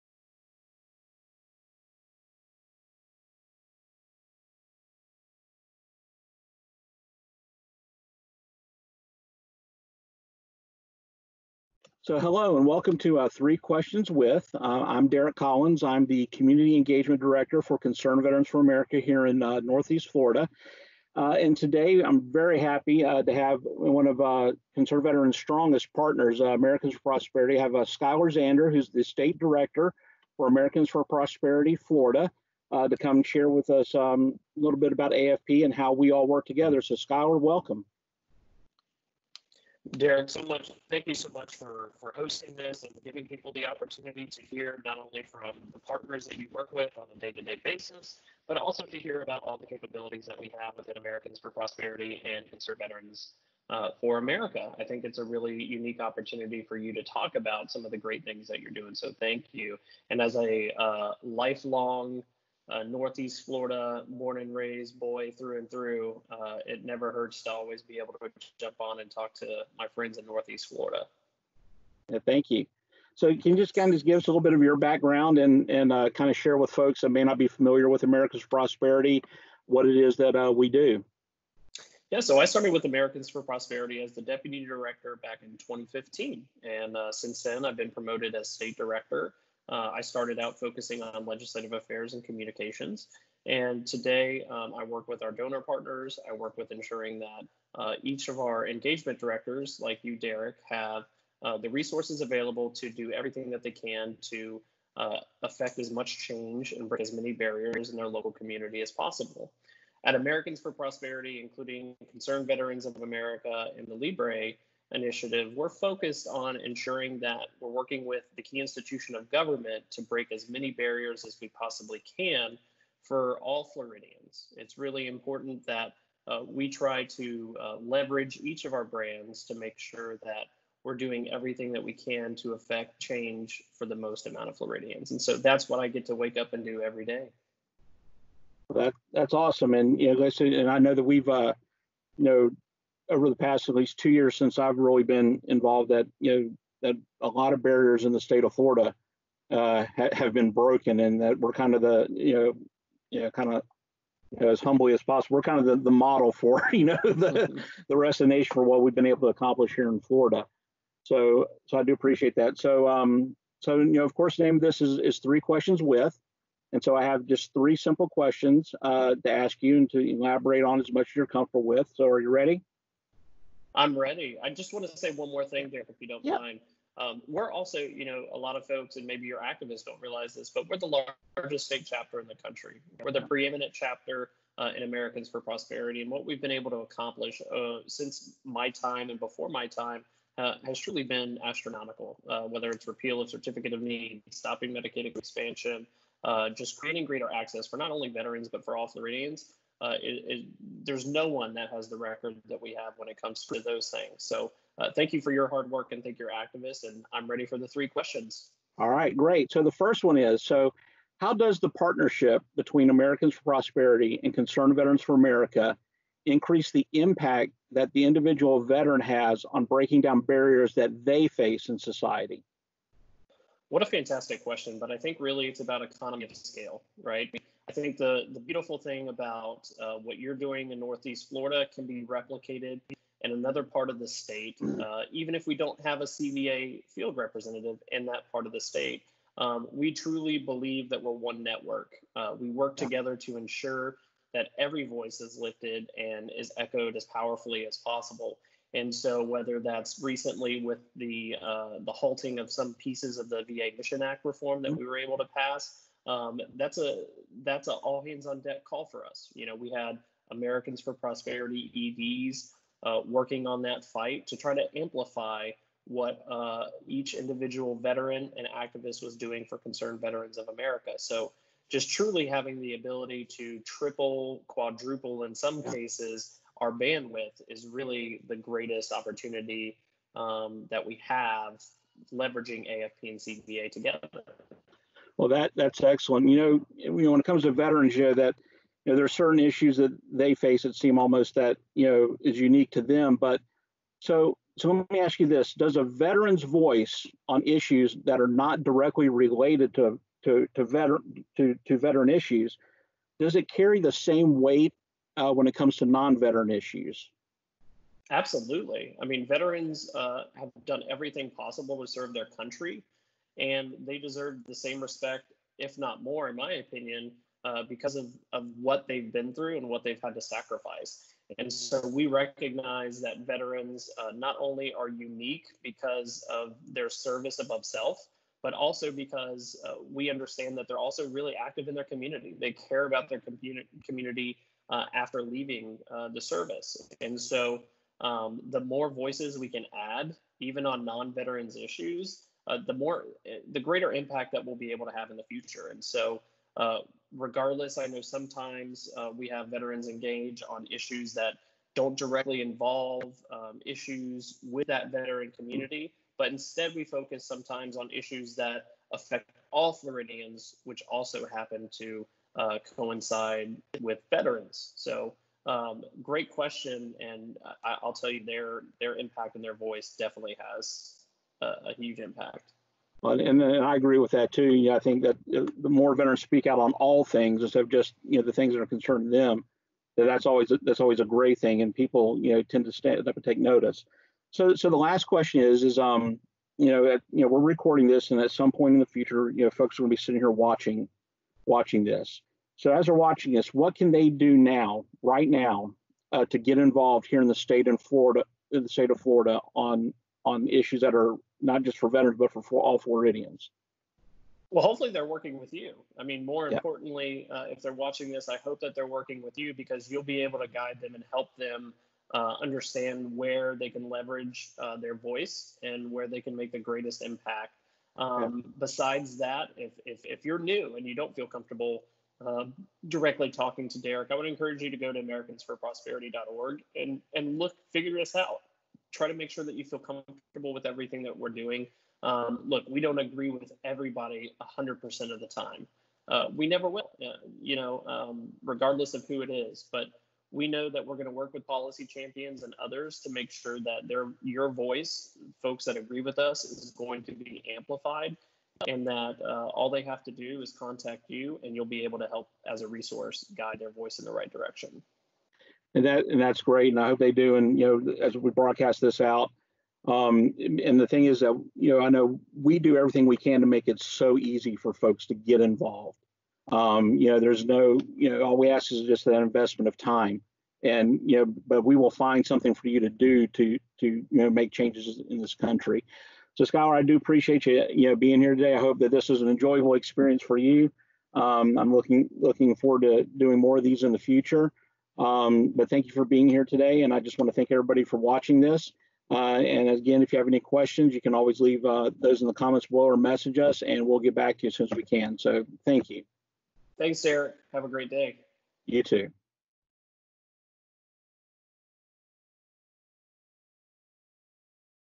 S1:E3 - 3 Qs w/Skylar Zander, AFP-FL State Director
CVA-FL Community Engagement Director Derek Collins sits down for a conversation with principled leaders on how their organizations are working to remove barriers for veterans and their communities so they can prosper and live better lives.